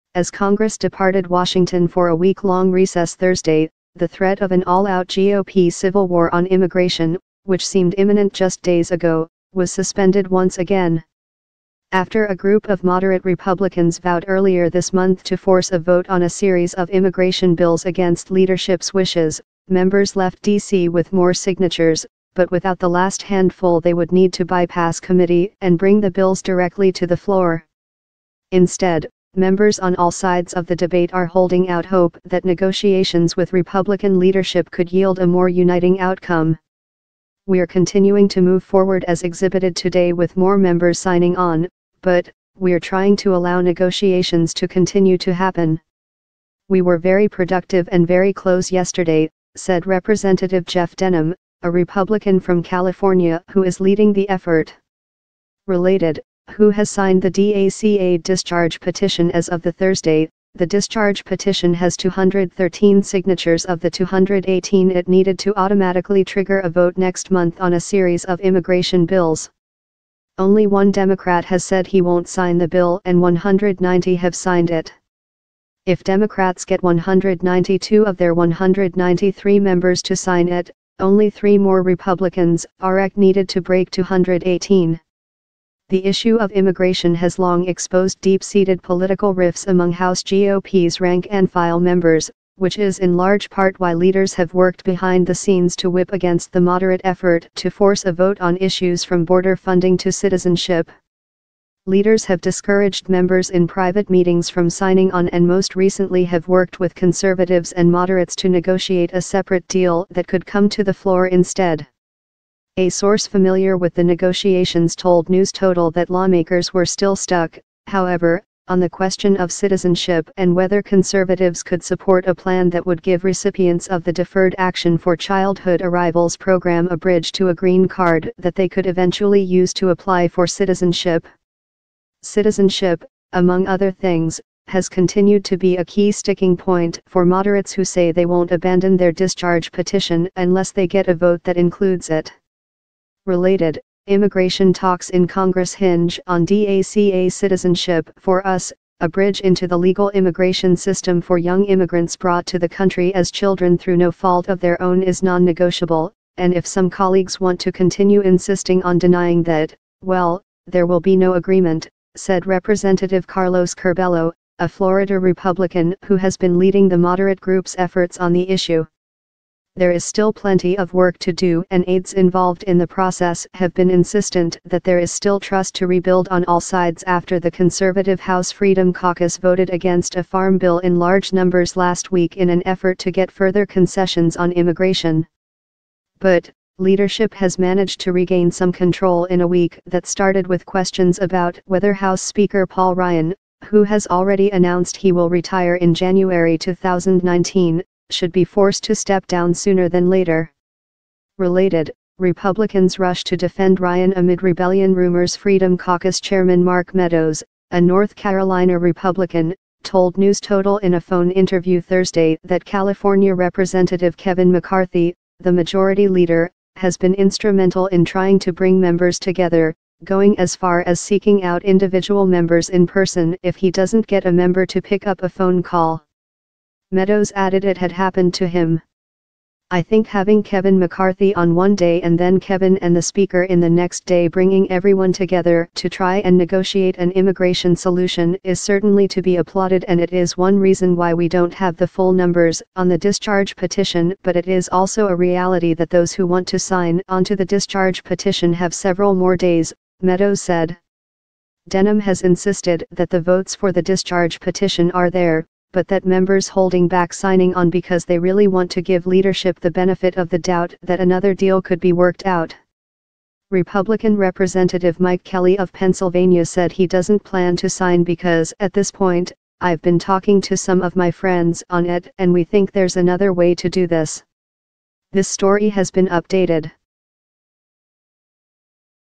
Republican Leaders Hold Off Coup On Immigration For Now Politics
As Congress departed Washington for a week-long recess Thursday, the threat of an all-out GOP civil war on immigration, which seemed imminent just days ago, was suspended once again.